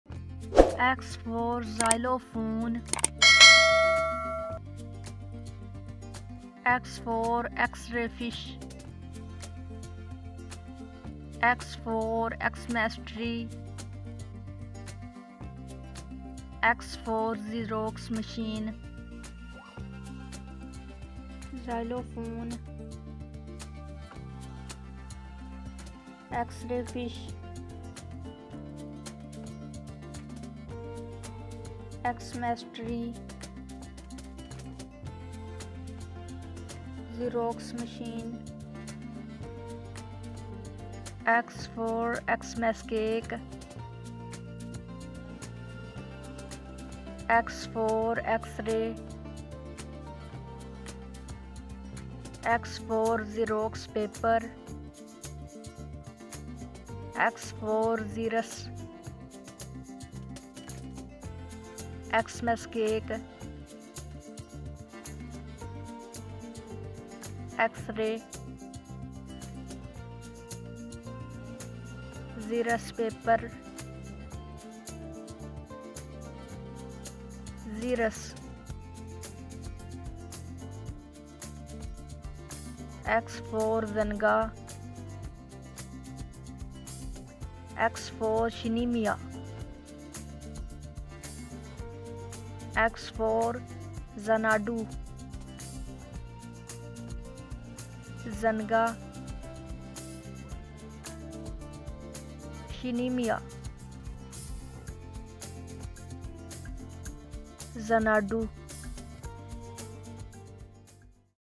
Learn letter X with Scope Kids | Xylophone, X-ray-fish , Xanga & Xmas cake
Welcome to Scope Kids, where learning
 the alphabet is an exciting adventure! Today, let's explore the letter X. First up, we have the xylophone, a musical instrument that produces beautiful sounds when struck with mallets. Next, we dive into the depths of the ocean to meet the extraordinary X-ray fish, known for its transparent skin that reveals its internal organs. Moving on, we encounter Xanga, a popular social networking platform where people can connect and share their interests. Lastly, we indulge in the delicious delight of Xmas cake, a traditional treat enjoyed during the festive season. Join us on this remarkable journey as we continue to discover the wonders of the alphabet with Scope Kids!